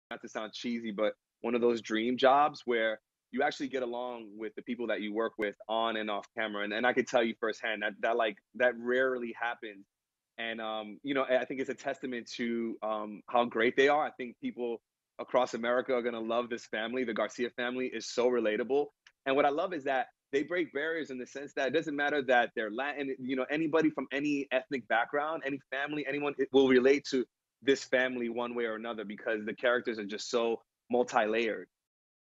For Victor Rasuk, It'd Be Beautiful If 'Beauty' Was A Hit
While it's starting out its life as a midseason replacement series, Victor Rasuk has high hopes for his show, The Baker and the Beauty. Some of that optimism comes from Rasuk's belief that it's a good, positive romantic comedy for our time, but he also admits some of it comes from a more selfish place -- he says he'd like to keep working with this cast and crew for a long, long time.

The Baker and the Beauty airs Mondays at 10/9c on ABC.